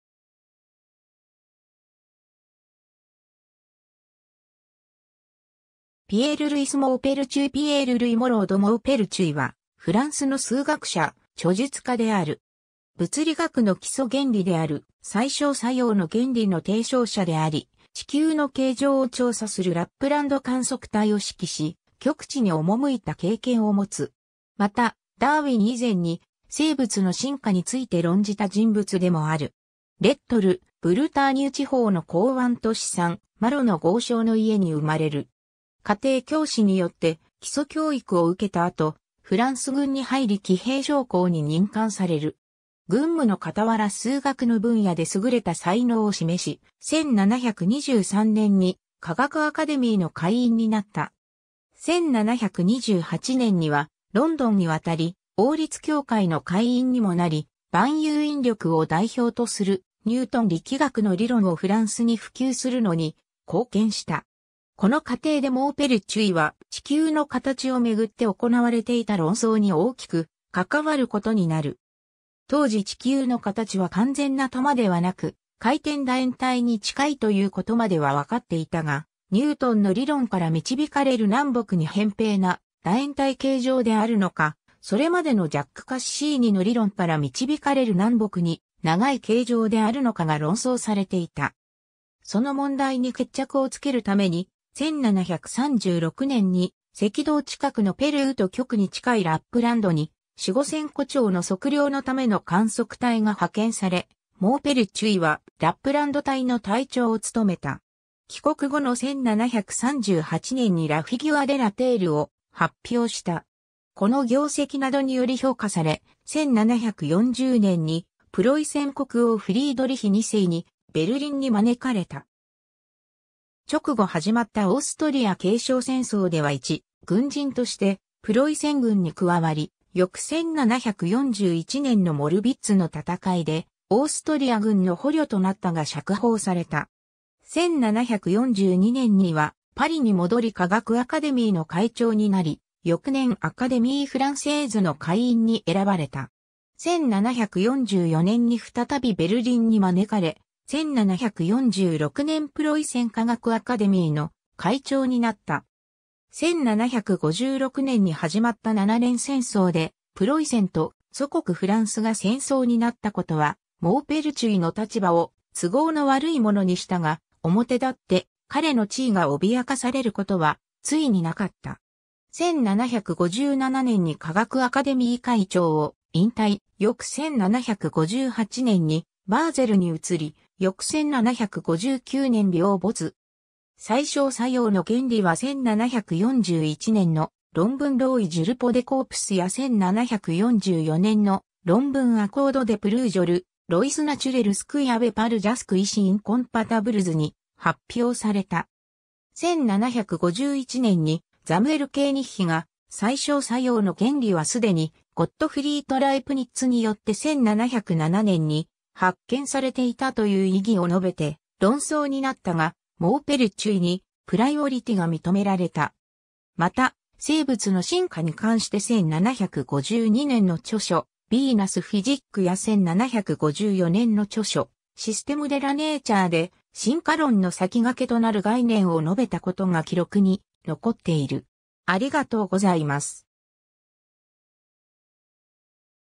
ピエール・ルイ・モーペルテュイ
アカデミー・フランセーズ会員
サン＝マロ出身の人物
Pierre Louis Maupertuis ピエール＝ルイ・モロー・ド・モーペルテュイ（Pierre-Louis Moreau de Maupertuis、1698年9月28日 – 1759年7月27日）は、フランスの数学者、著述家である。
物理学の基礎原理である最小作用の原理の提唱者であり、地球の形状を調査するラップランド観測隊を指揮し極地に赴いた経験を持つ。
またダーウィン以前に生物の進化について論じた人物でもある。
 Lettres ブルターニュ地方の港湾都市サン・マロの豪商の家に生まれる。
家庭教師によって基礎教育を受けた後、フランス軍に入り騎兵将校に任官される。
軍務の傍ら数学の分野で優れた才能を示し、1723年に科学アカデミーの会員になった。
 1728年にはロンドンに渡り王立協会の会員にもなり、万有引力を代表とするニュートン力学の理論をフランスに普及するのに貢献した。
この過程でモーペルテュイは地球の形を巡って行われていた論争に大きく関わることになる。
 当時地球の形は完全な球ではなく回転楕円体に近いということまでは分かっていたが、ニュートンの理論から導かれる南北に扁平な楕円体形状（扁球）であるのか、それまでのジャック・カッシーニの理論から導かれる南北に長い形状（長球）であるのかが論争されていた。
その問題に決着をつけるために、1736年に赤道近くのペルーと極に近いラップランドに子午線弧長の測量のための観測隊が派遣され、モーペルテュイはラップランド隊の隊長を務めた。
帰国後の1738年にLa figure de la terre（『地球の形状』）を発表した。
 この業績などにより評価され、1740年にプロイセン国王フリードリヒ2世にベルリンに招かれた。
直後始まったオーストリア継承戦争では一軍人としてプロイセン軍に加わり翌1741年のモルヴィッツの戦いでオーストリア軍の捕虜となったが釈放された。
 1742年にはパリに戻り科学アカデミーの会長になり、翌年アカデミー・フランセーズの会員に選ばれた。
 1744年に再びベルリンに招かれ、1746年プロイセン科学アカデミーの会長になった。
1756年に始まった七年戦争でプロイセンと祖国フランスが戦争になったことはモーペルテュイの立場を都合の悪いものにしたが、表立って彼の地位が脅かされることはついになかった。
1757年に科学アカデミー会長を引退、翌1758年にバーゼルに移り翌1759年病没。
 最小作用の原理は1741年の論文"Loi du repos des corps"や1744年の論文"Accord de plusieurs lois naturelles qui avaient paru jusqu’ici incompatibles"に発表された。
1751年にザムエル・ケーニッヒが最小作用の原理はすでにゴットフリート・ライプニッツによって1707年に発見されていたという異議を述べて論争になったが、モーペルテュイにプライオリティが認められた。
 また生物の進化に関して1752年の著書"Venus physique"や1754年の著書"Systeme de la Nature"で、進化論の先駆けとなる概念を述べたことが記録に残っている。